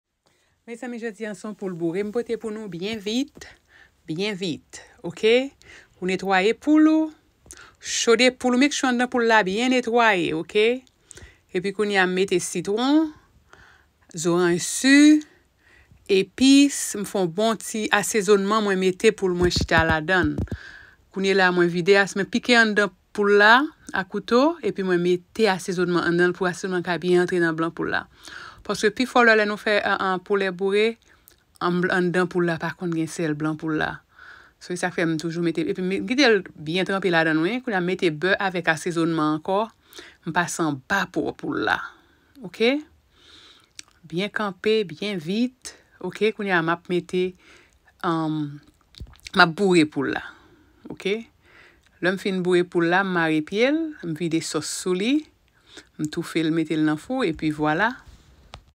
Rotisserie Chicken Haitian Style
Ingredients
1 whole chicken
3 potatoes
2 carrots
1 onion
1/2 cup of butter
2 tablespoons of lime juice
Adobo seasoning to taste
Haitian Épis seasoning
Oil